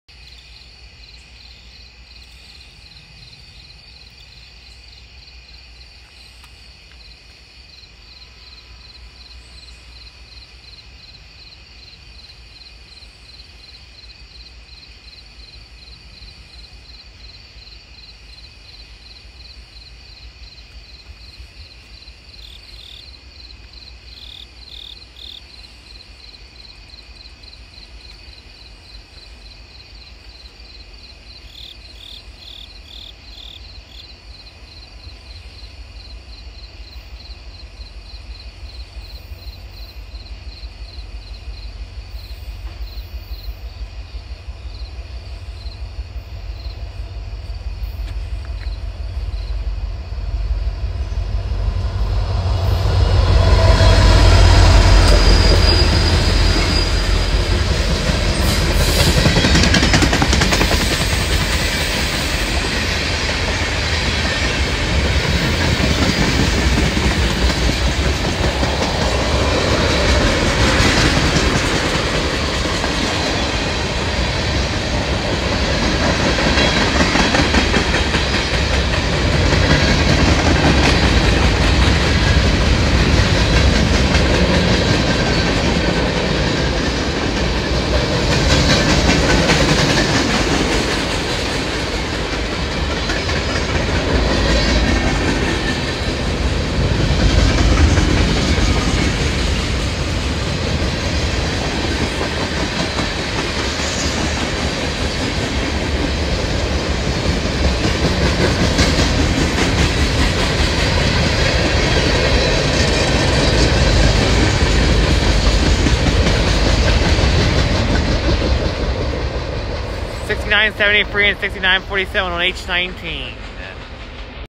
Norfolk Southern H19 - 9/13/22
Norfolk Southern local freight train H19 is seen at Second Street on the Port Road Branch in Saginaw, Pennsylvania at 9:05 PM on Tuesday, September 13, 2022. The engines in this video are: NS 6973 (SD60E) and NS 6947 (SD60E). Enjoy!